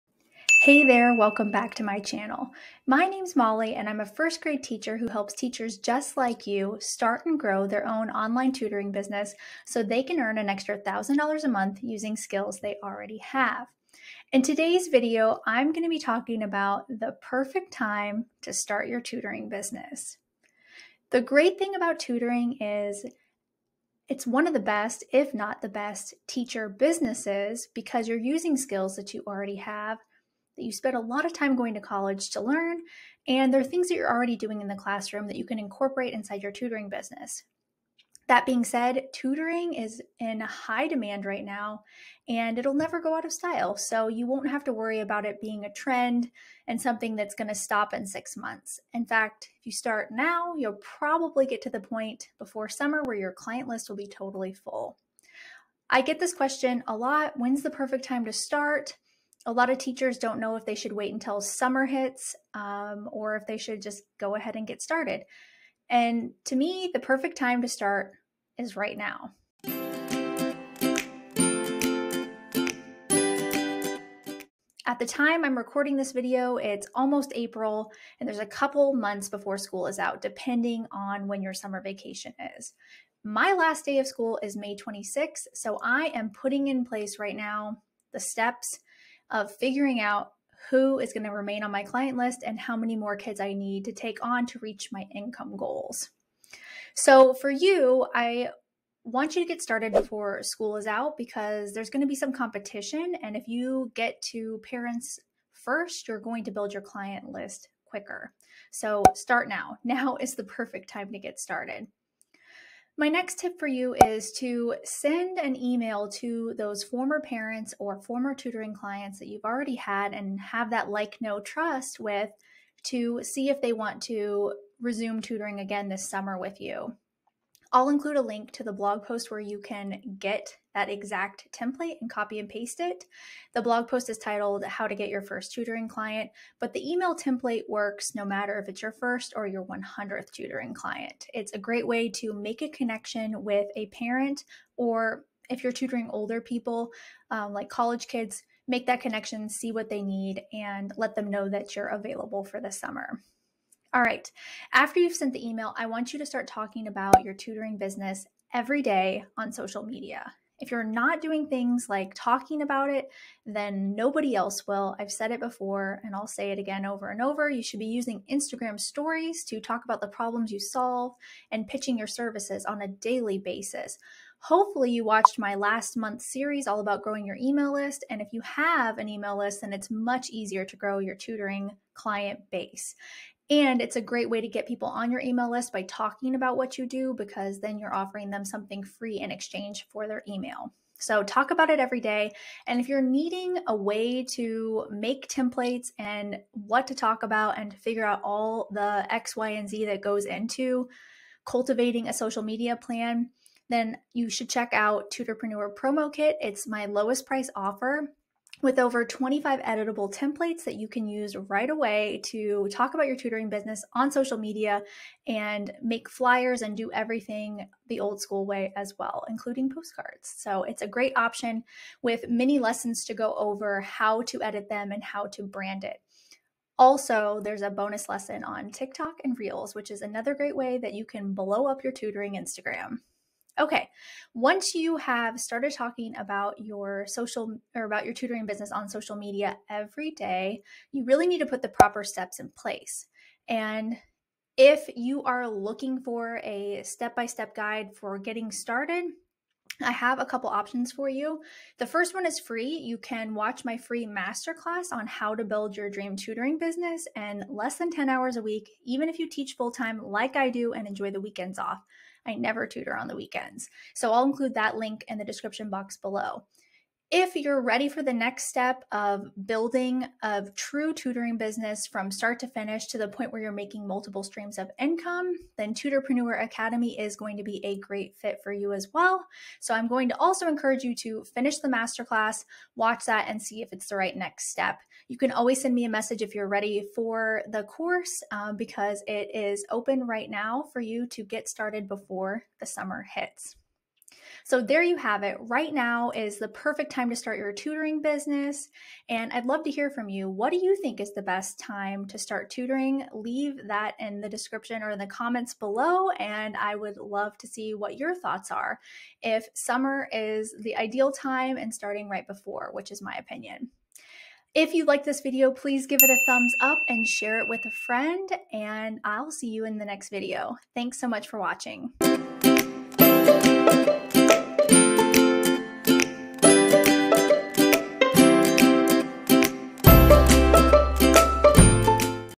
The Perfect Time to Start Your Tutoring Business
You may know that you want to start your tutoring business, but do you know when? I’m going to tell you all about the perfect time to start your tutor business and why.